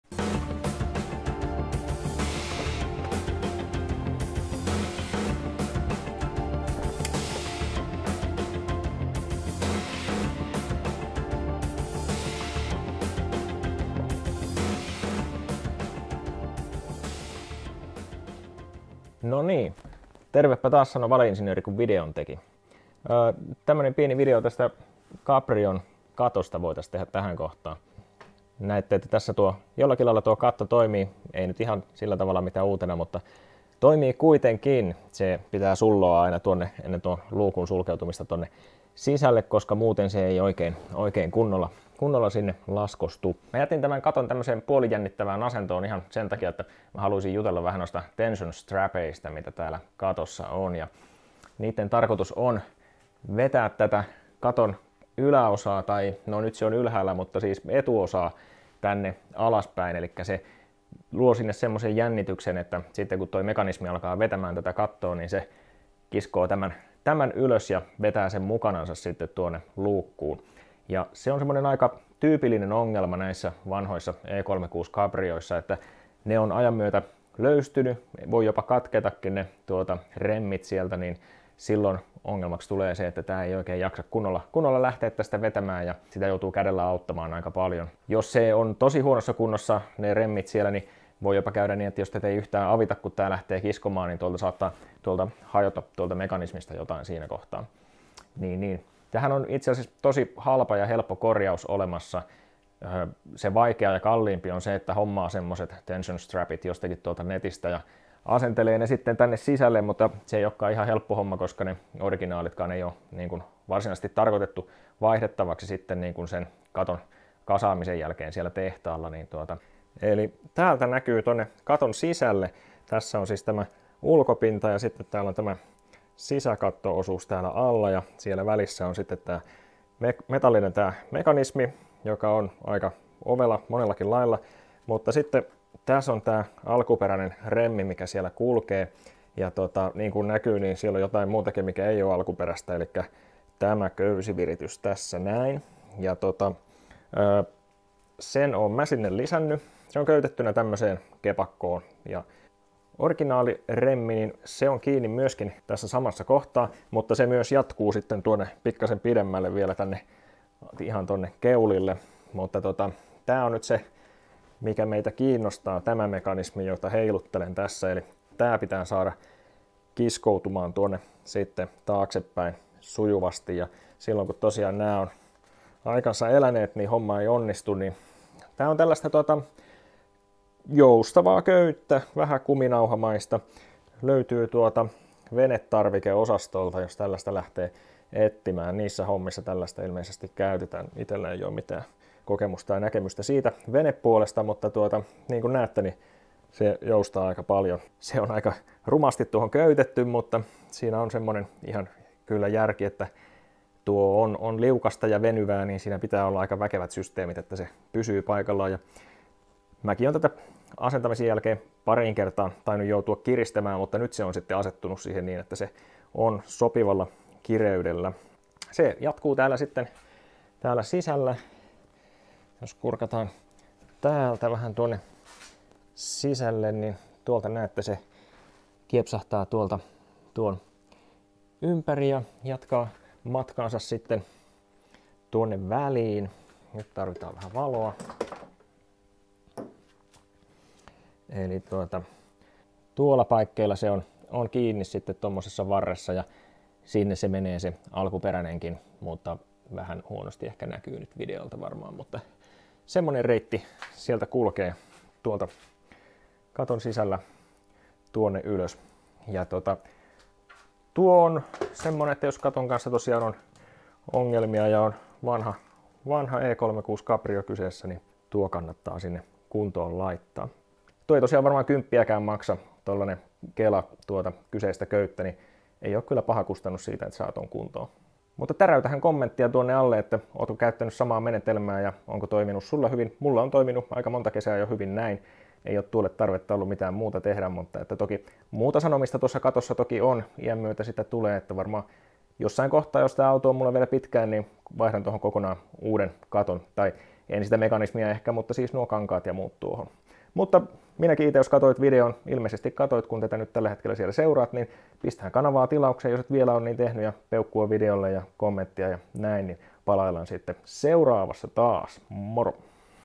Vanhuuden oireita // E36 Cabrio Tension strap -korjaus
BMW E36 Cabrion tension strapit löystyvät vanhuuttaan. Alkuperäisten katon remmien vaihtaminen samanlaisiin on vähän hankalaa, mutta homman voi onneksi toteuttaa paljon helpommalla ja halvemmalla. Omatekoinen köysiviritys ei ehkä ole kaunis, mutta eipä se sieltä katon välistä mihinkään näy.